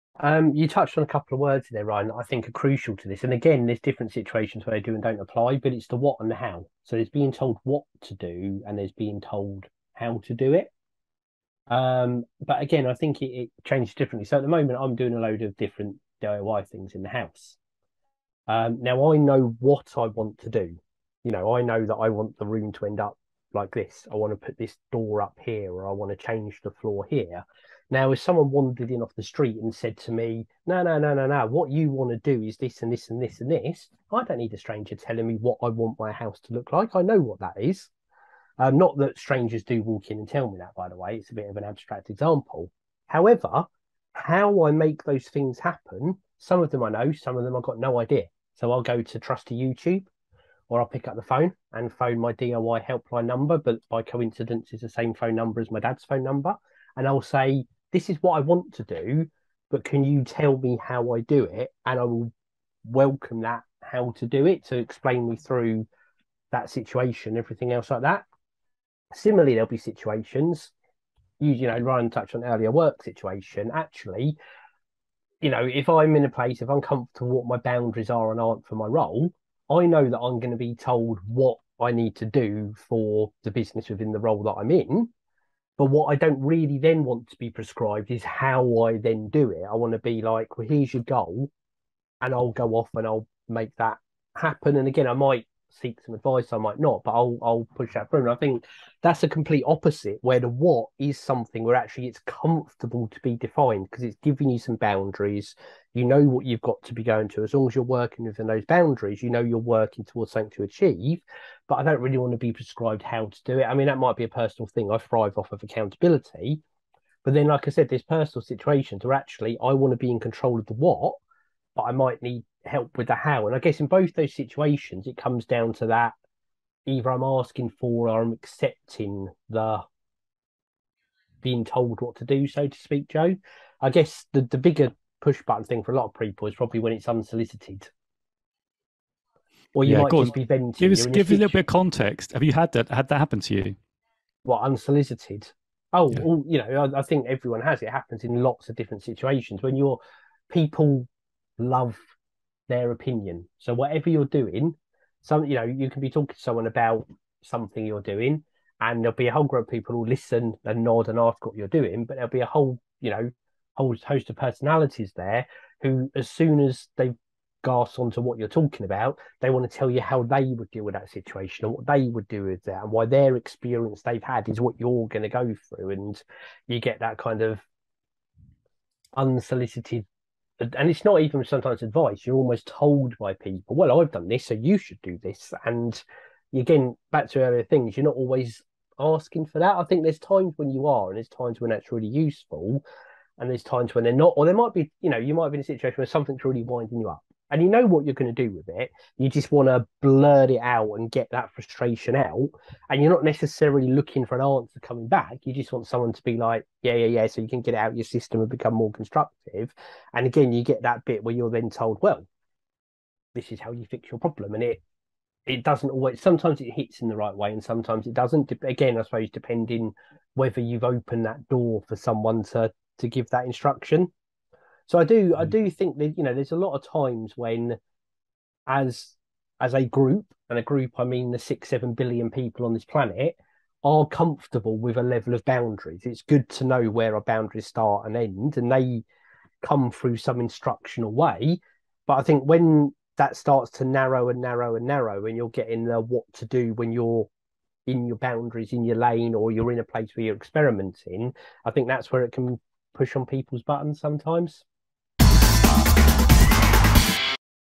Difference between HOW and WHAT
Difference between the how and the what, in this episode (episode 181)  Lee explains how advice in this content can either motivate or de-motivate you to take advice.  This is pure gold!!

Much love